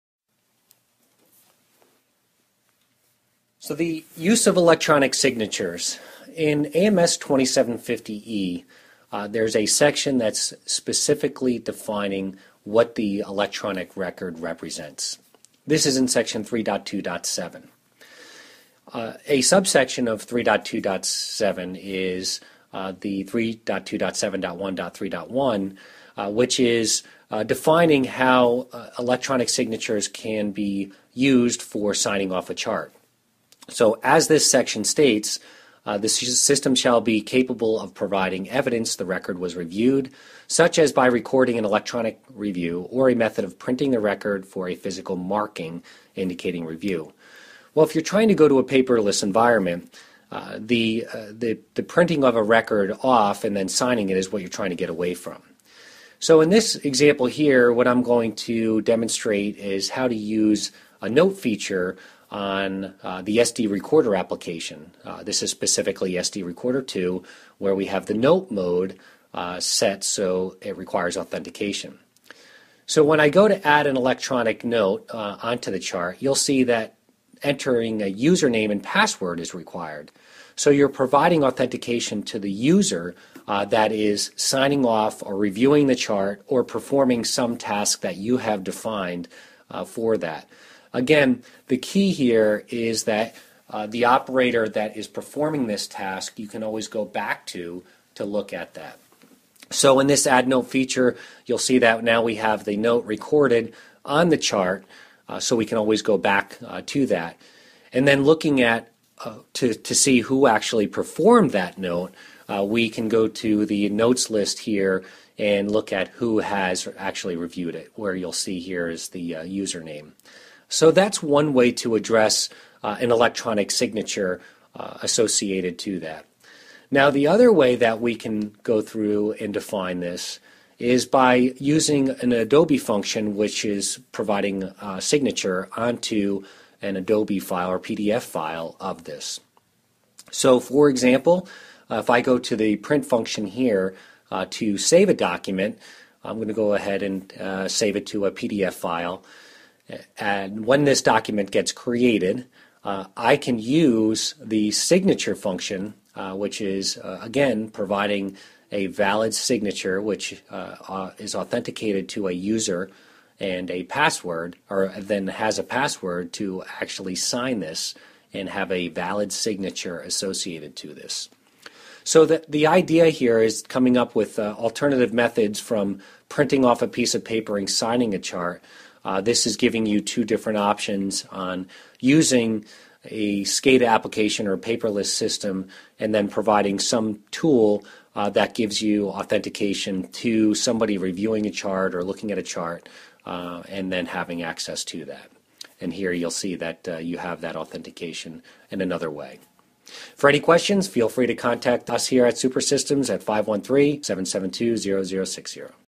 Heat Treatment Tip: Electronic signatures for AMS2750 E using Super Systems software
Jim Oakes, SSi US, provides a very useful update on AMS 2750 E and Super Systems innovative approach to entering an electronic signature on our heat treatment specific sCADA/HMI software package.